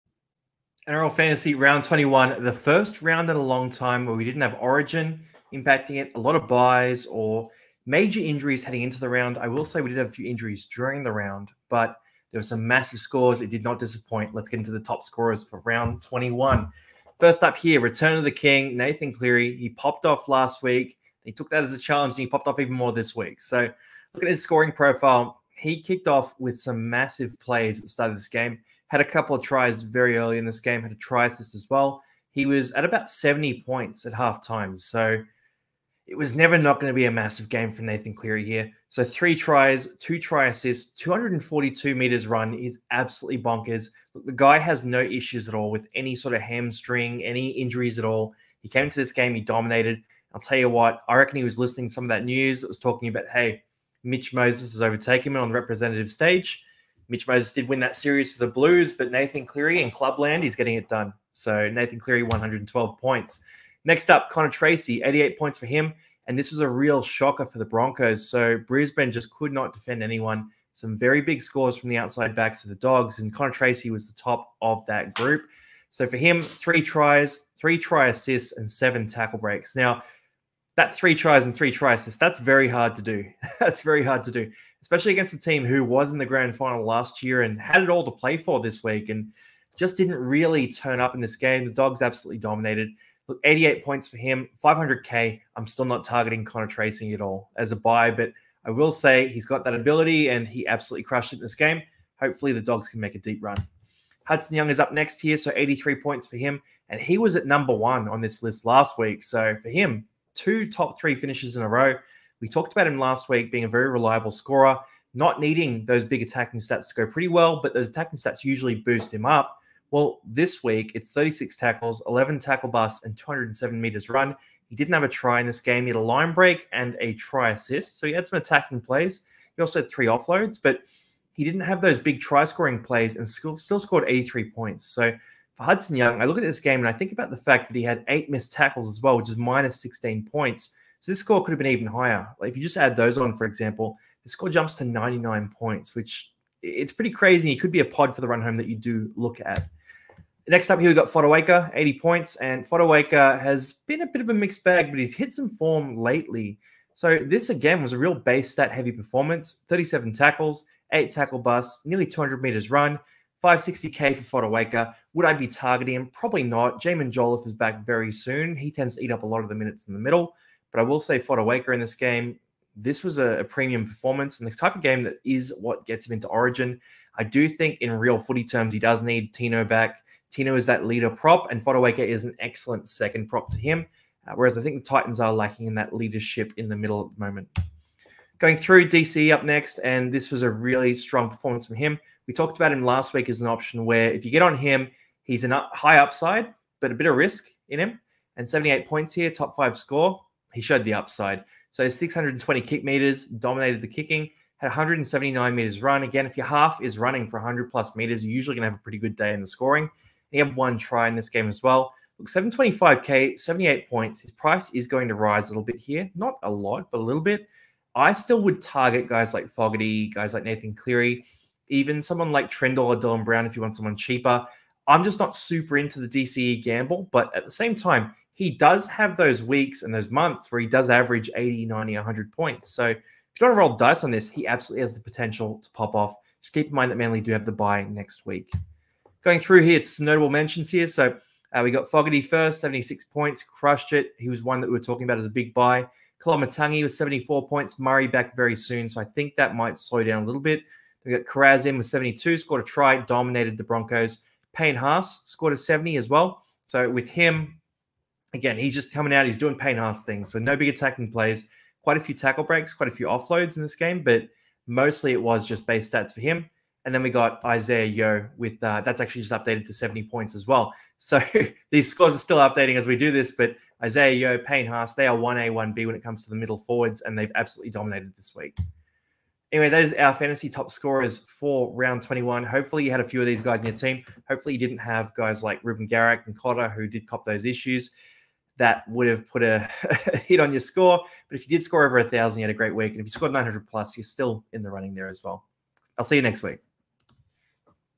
NRL FANTASY: Round 21 TOP SCORERS
Longy AKA The Casual Athlete runs you through the top scorers from Round 21 of NRL Fantasy.